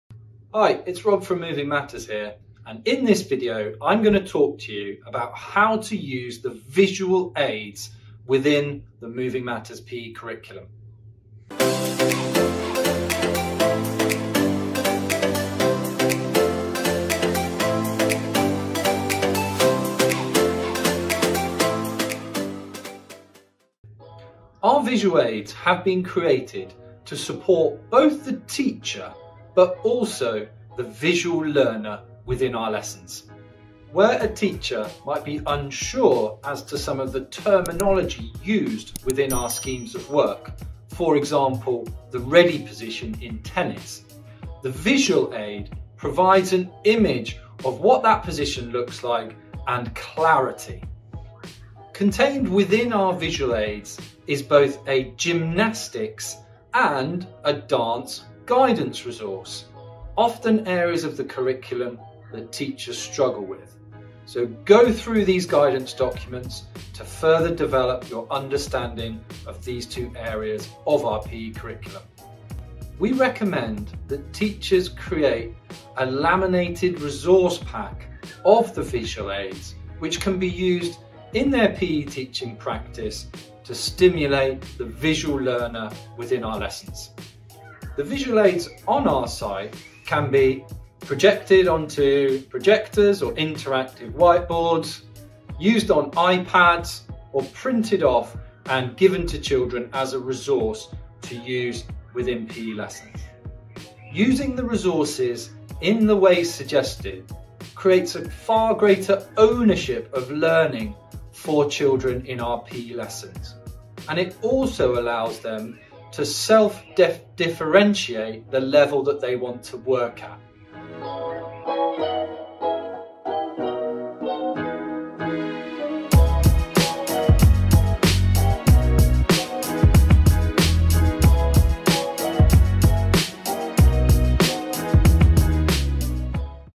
How to Teach Primary PE: Using Visual Aids
In the latest in our How to Teach Primary PE series, we look at the use of visual aids in PE lessons, how they support high quality teaching and learning and the visual learners in our groups.